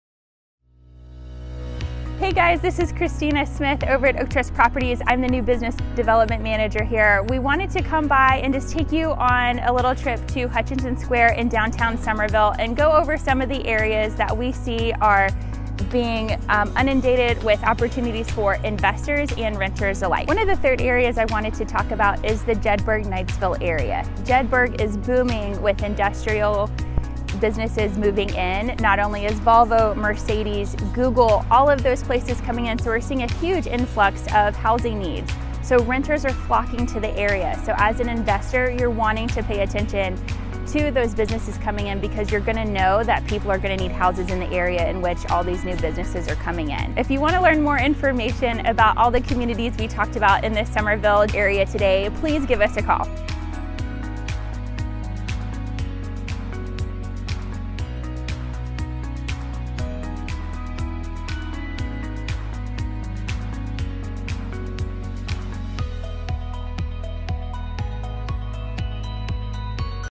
Best Areas To Invest In Charleston: Jedburg
We have been covering the best places for investors to grow their portfolios within the Charleston area this month. So far we have discussed Nexton and Cane Bay Plantation.

Today we are here to discuss the Jedburg area!

Our team is ready to help you with your investment journey, and we are ready for your call!